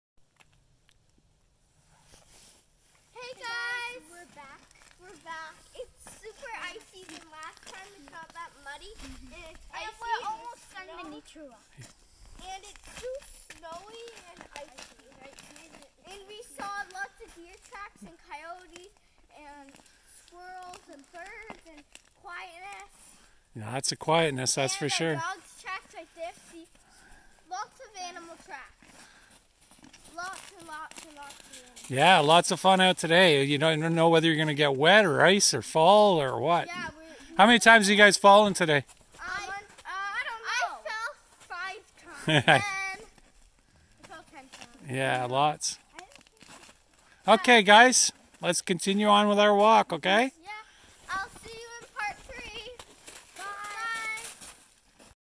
Nature Walk Part 2
This is the second part of our nature walk with our OMG dolls.